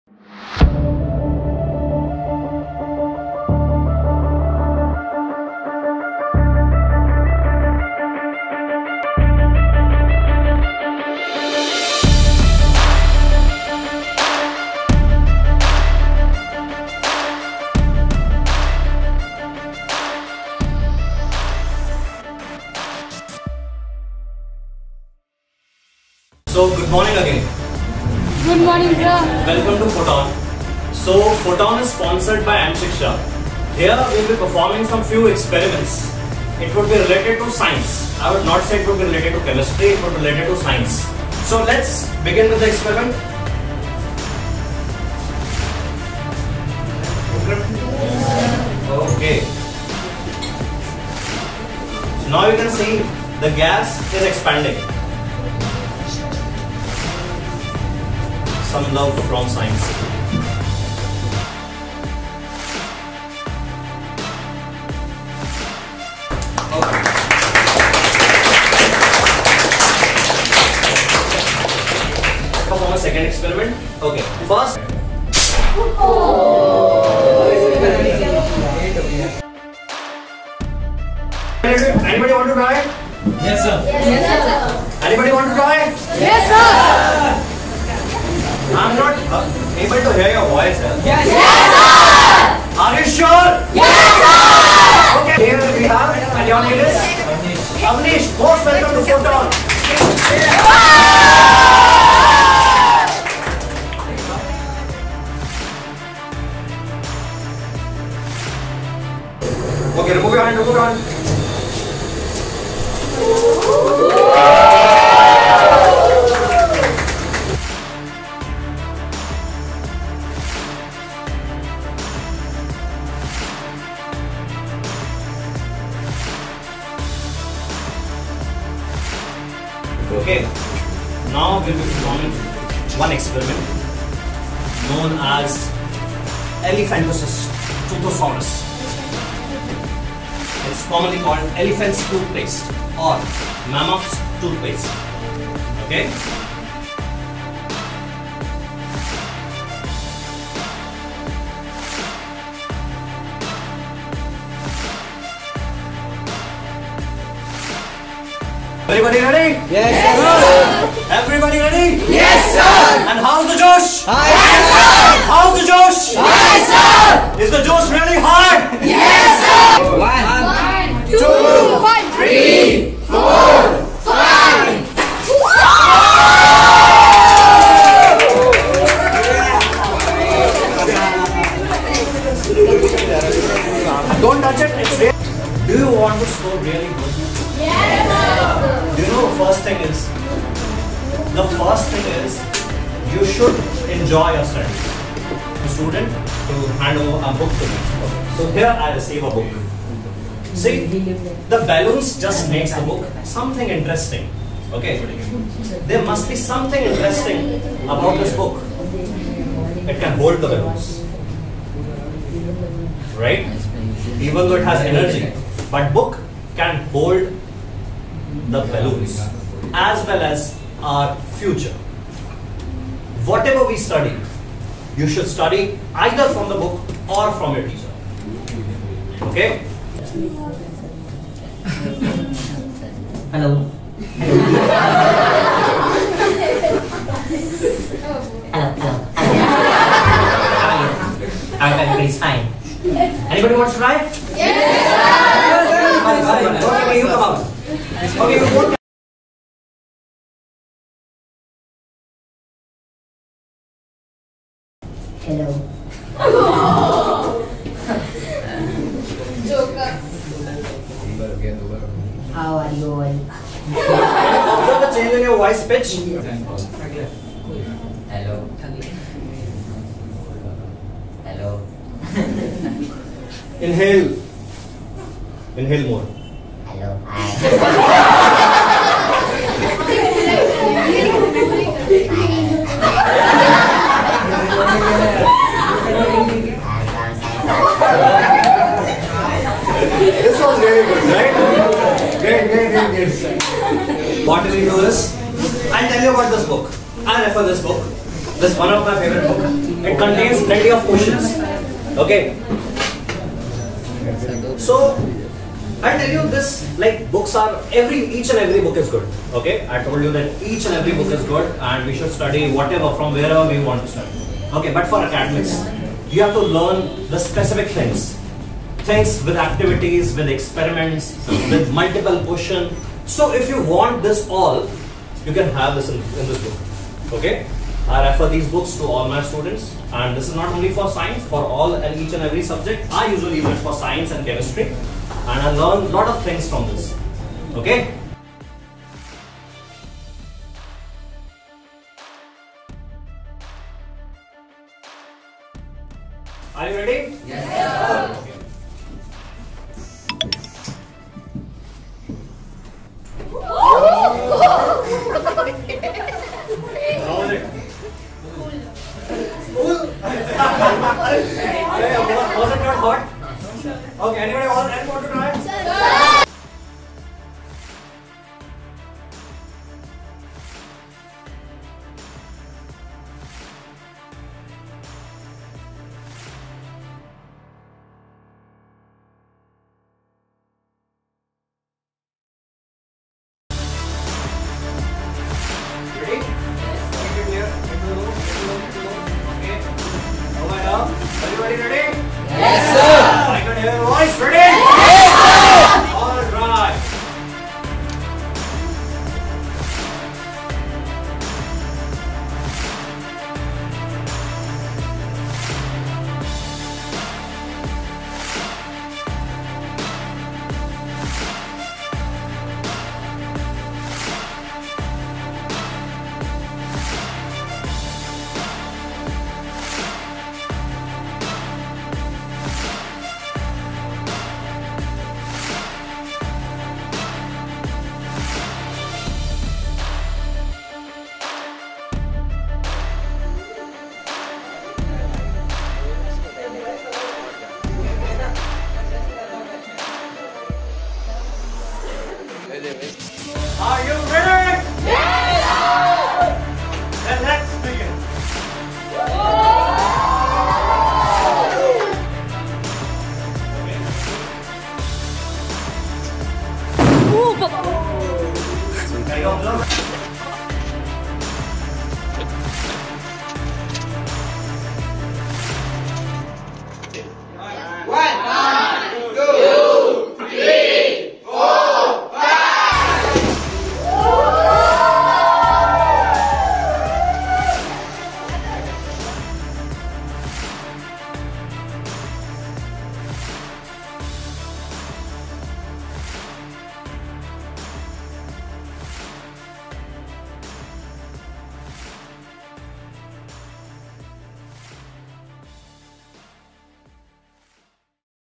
PHOTON | SCIENCE EVENT | POWERED BY - M SHIKSHA | 7 AUG 2022
Science is not only cool, it's also a smart investment for your future. See why here
Here we perform live science experiment that’s both entertaining and educational.
Photon is an science event for students. It's designed to develop interest in science and technology by encouraging students through live experiments, while also offering a fun, engaging ways to learn about science.
Chapters
00:00 PHOTON SCIENCE EVENT
00:42 DRY IC BUBBLES
01:23 MAGNESIUM RIBBON BURNING
02:19 ELEPHANT TOOTHPASTE
03:50 VOICE CHANGE USING HELIUM
05:41 BUTANE BURNING BUBBLES
06:45 SALT COLOUR FLAME
07:13 RED FLAME (LITHIUM SALT)
07:25 GREEN (COPPER SULPHATE)
07:37 BURNING HYDROGEN
07:49 HEAVEN CLOUD (LIQUID NITROGEN)
08:18 END